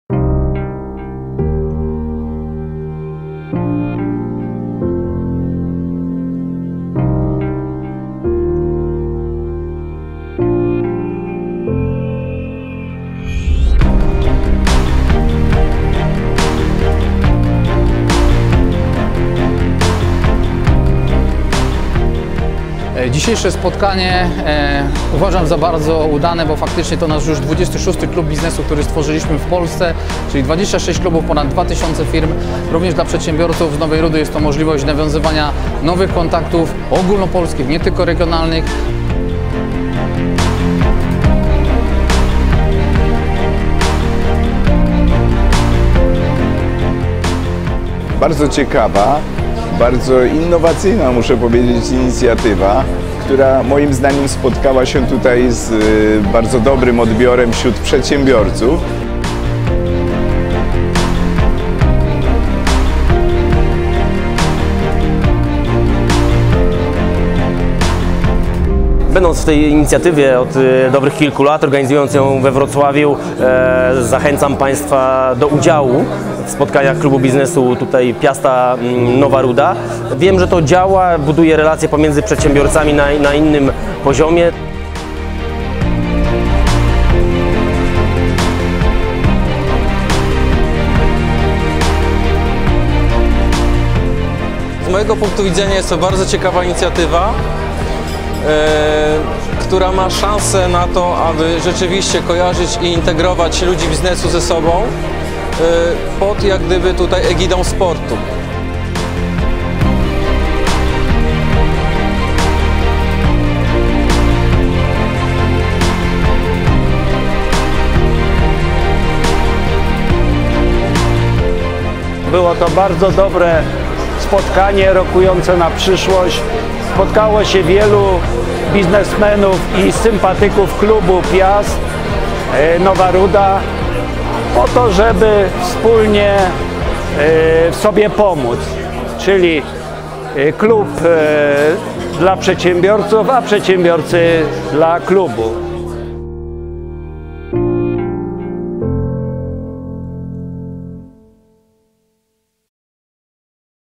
Ruszył projekt Piast Biznes Klub
Przy drużynie Piast Nowa Ruda został powołany do życia projekt, który w kraju cieszy się  dużą popularnością, a mianowicie Klub Biznesu, którego celem jest wsparcie klubu sportowego oraz pomoc przedsiębiorcom w rozwijaniu ich działalności.